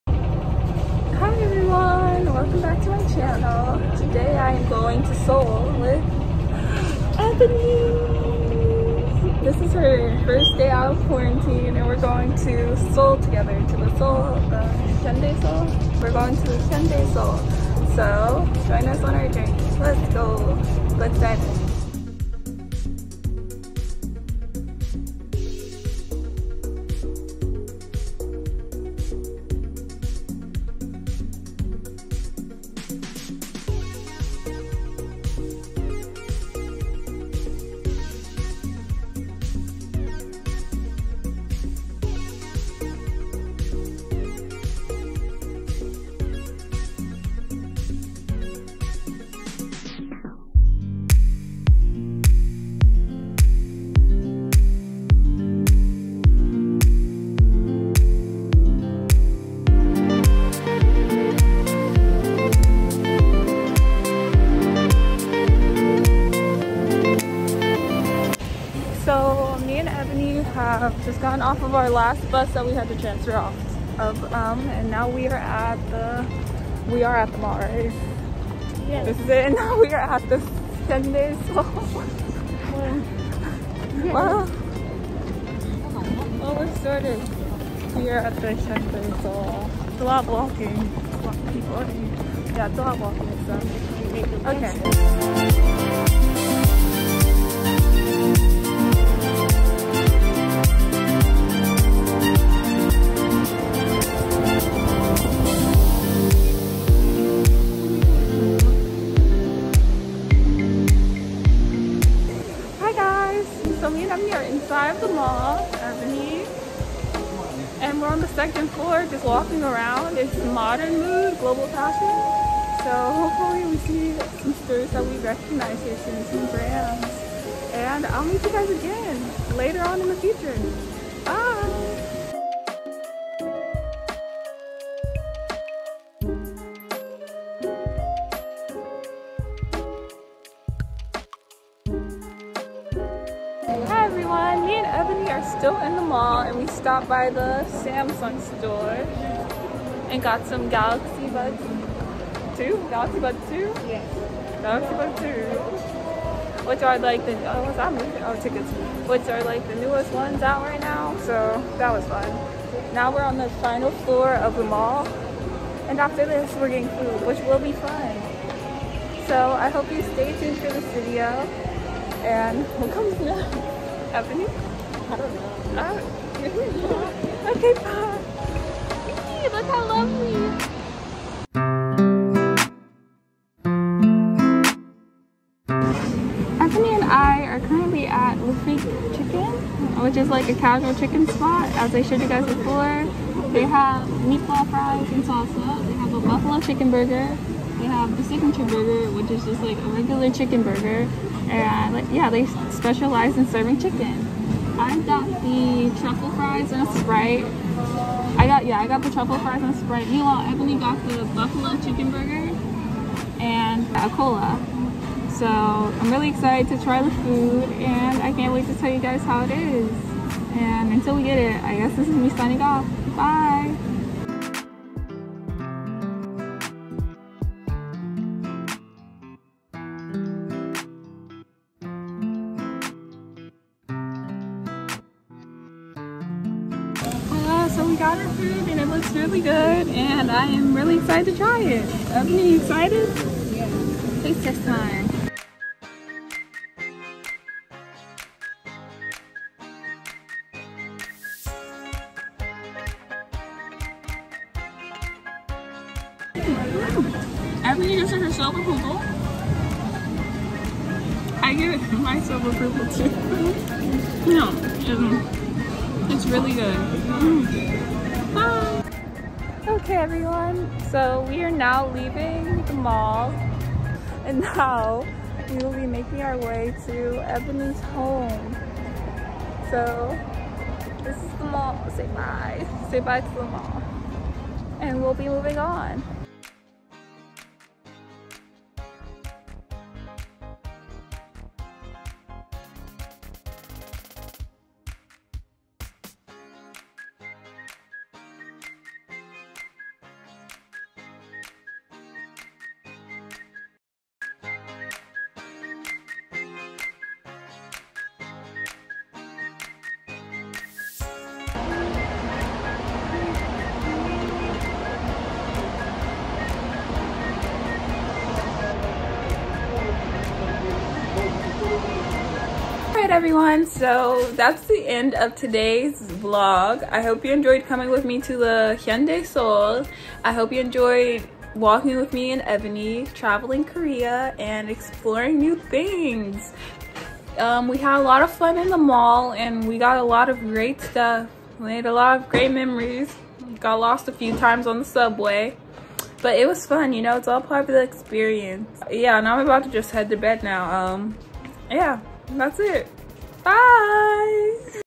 trip to the hyundai seoul! | study abroad | dankook university
today my sister (@ebony_shirell) and I took a trip to the the hyundai seoul, a super beautiful mall in seoul! it was filled with a bunch of various luxury stores and we really enjoyed walking around! 🤍